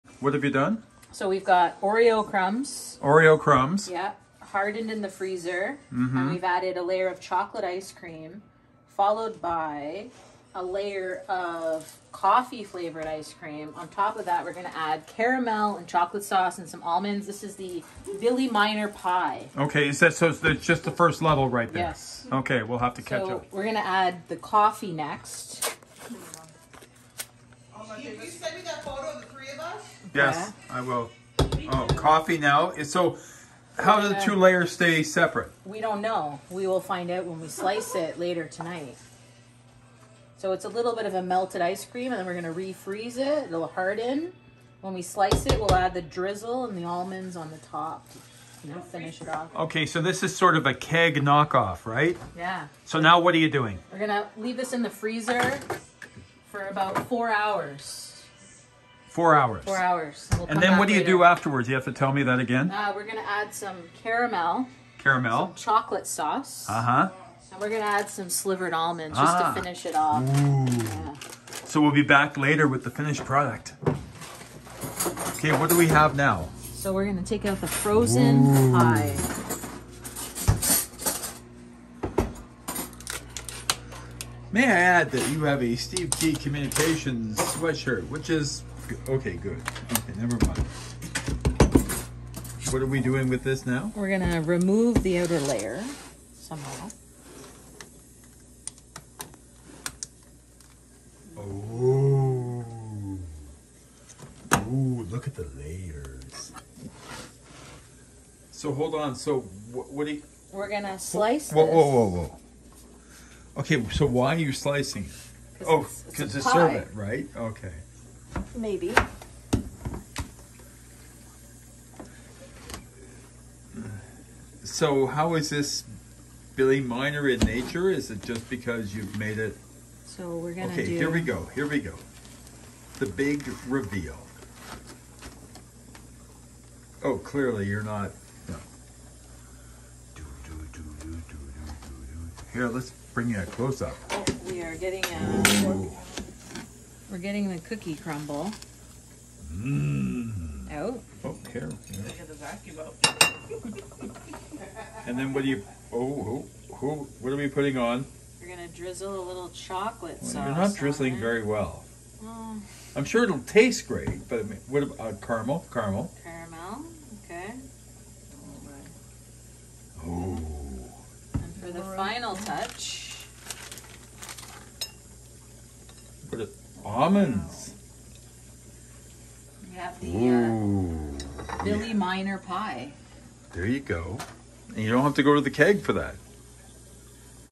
2022-01-24 kee2travel - Cooking with Kelley — home made Billy Miner Pie
Screw you The Keg.  :)  We made this for a fraction of the cost and you know something…it was better.